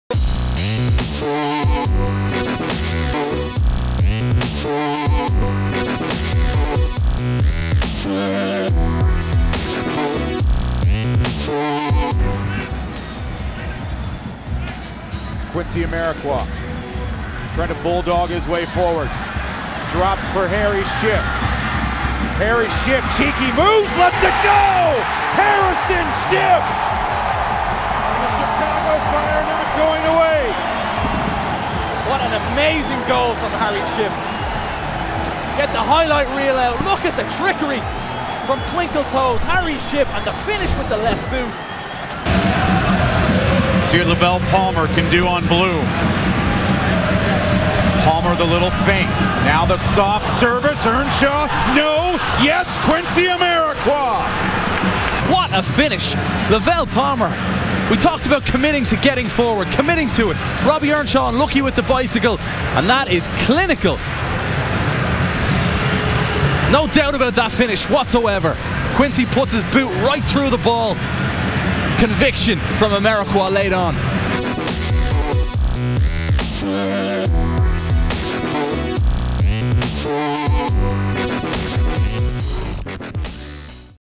2014 Aldi Goal of the Year | Day 11: #2 Harry Shipp vs. #1 Quincy Amarikwa
The 2014 Aldi Chicago Fire Goal of the Year continues in the Nowak bracket with "twinkle toes" Harry Shipp's dance around Seattle defenders vs. Quincy Amarikwa's classy finish.